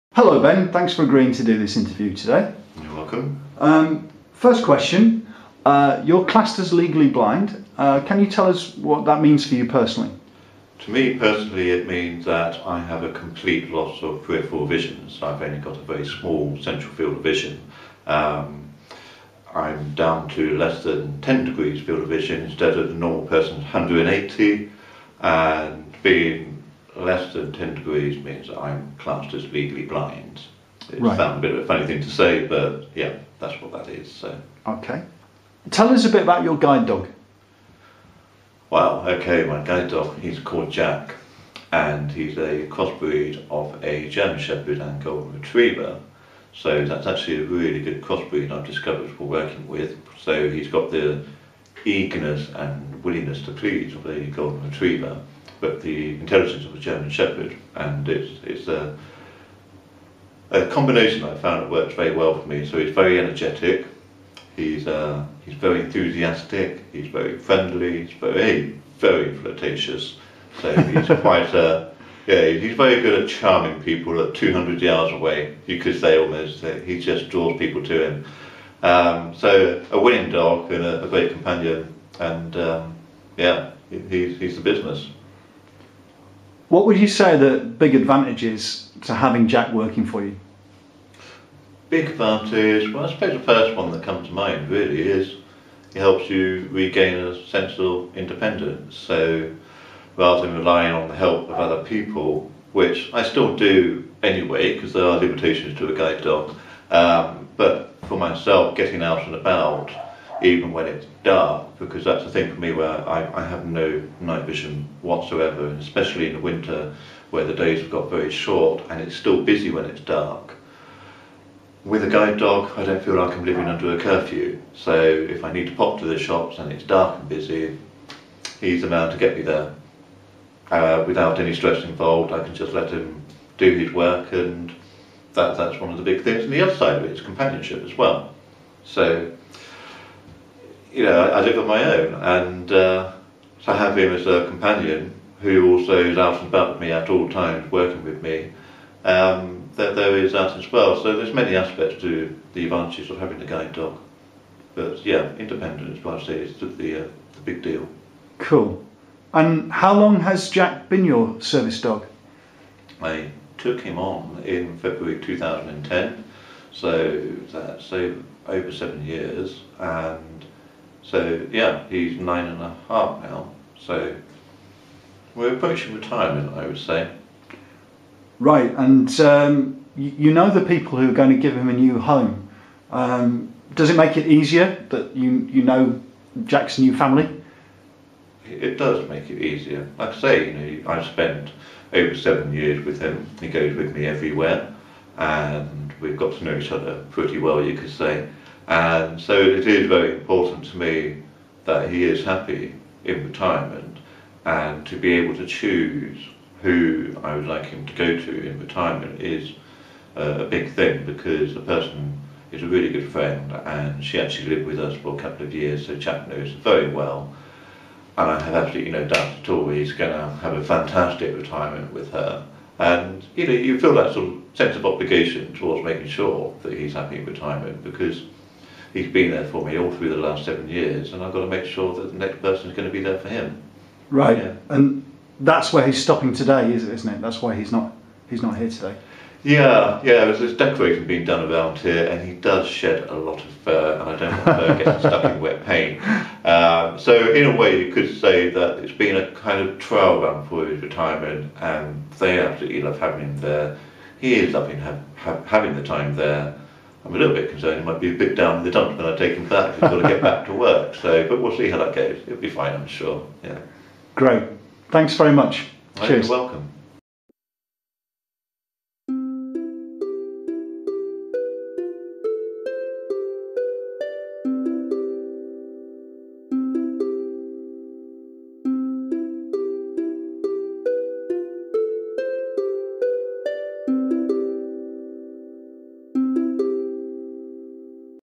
Life with a Guide Dog: An Interview
A short interview with Ben about life with his guide dog Jack.

Jack is a Golden Retriever / German Shepherd cross whose intelligence, enthusiasm and energy has boosted Ben's independence, and provided valued companionship.
After 7 years as Ben's working service dog, it's time to start thinking about his well-earned retirement - Ben talks about the person who will provide Jack's new home at some point in the future.

Jack couldn't join us for interview today, because he was staying in his future retirement home temporarily, while painting & decorating work was being done in the house - as Ben says: Jack does shed quite a bit of fur, which can end up in the paint!